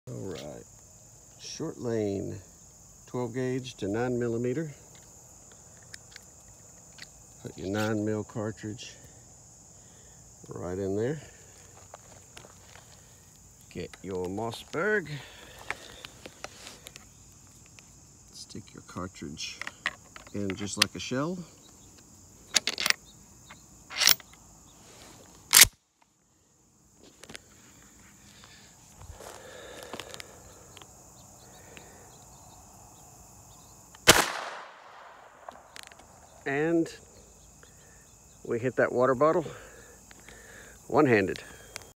Short Lane gun adapter 12ga to 9mm shotgun to pistol
Stainless steel conversion for mossberg pump shotgun to pistol cartridge ammo.